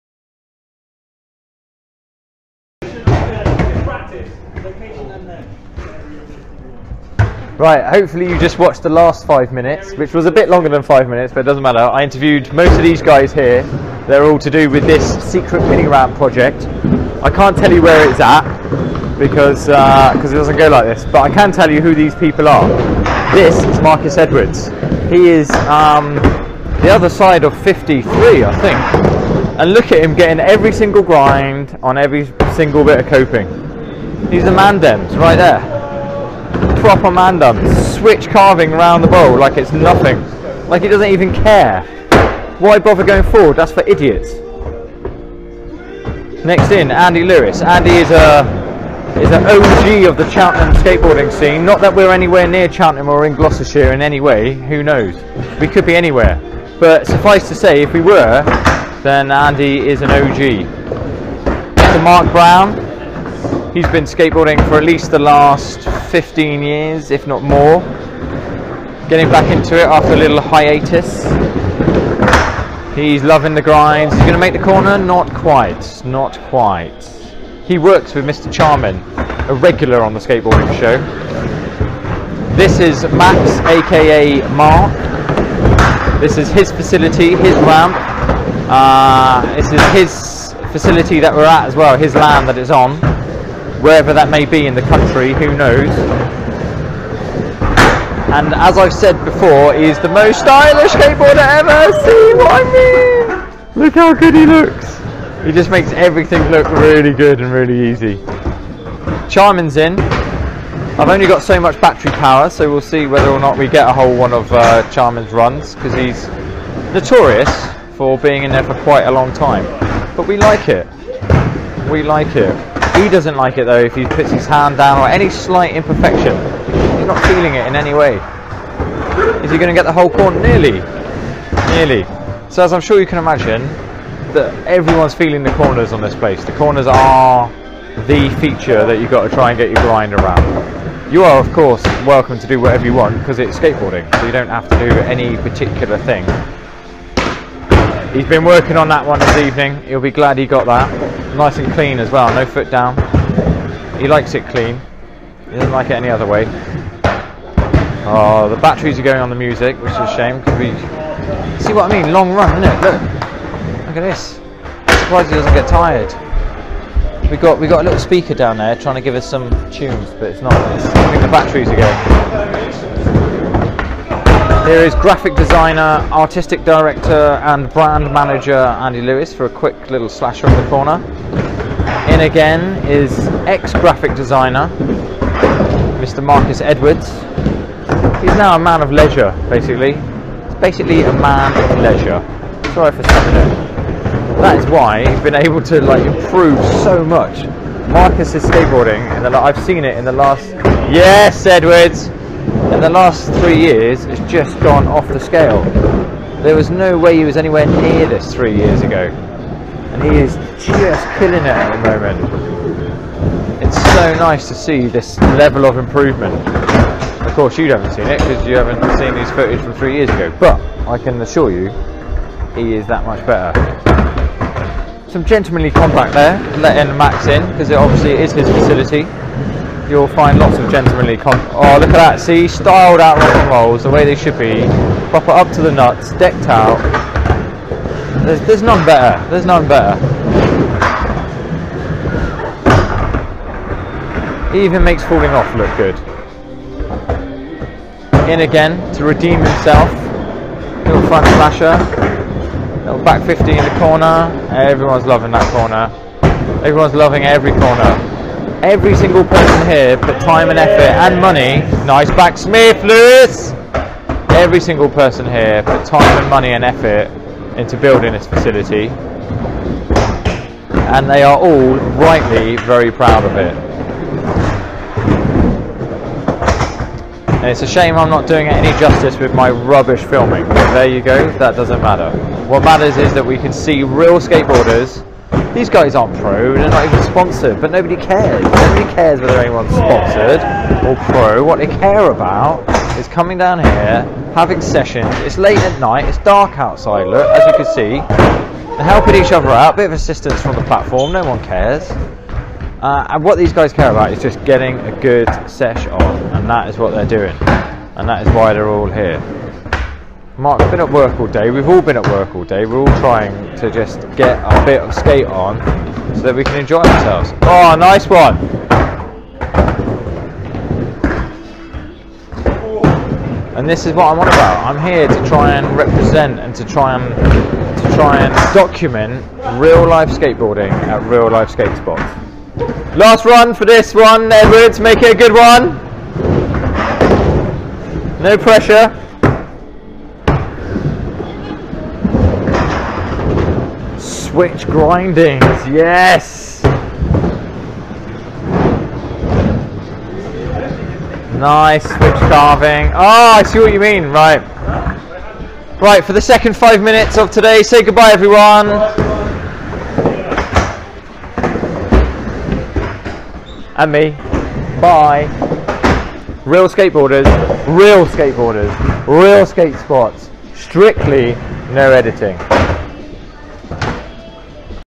5 Minutes... SMRP Session
After all the questions had been answered then it was time to get some skateboarding going!